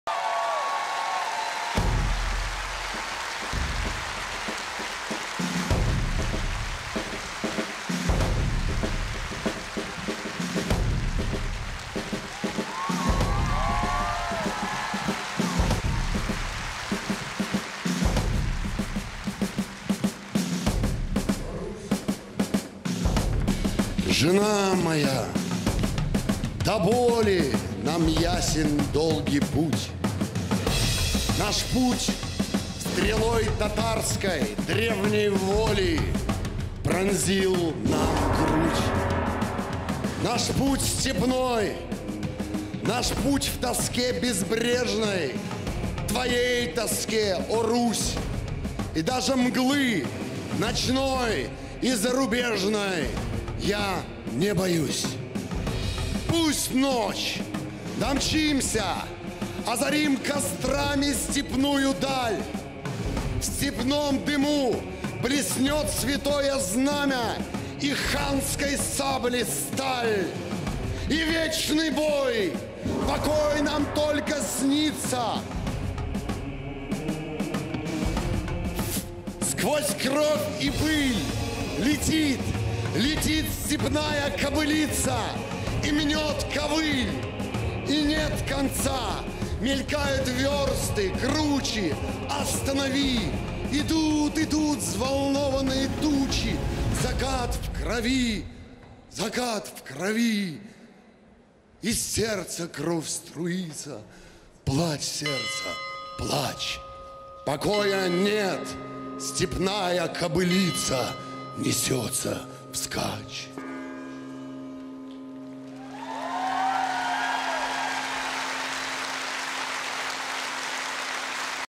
From Rus' to Russia - 10 - On Kulikovo Field
Day of Russia 2015, a grand concert on the Red Square

"On Kulikovo Field"
Gosha Kutsenko
fragment of poem by A. Blok
[translation by A. Wachtel, I. Kutik and M. Denner]

"Silver wolf" retinue,
interclub association "Moscow shooters",
drummer group "Vasiliev Groove"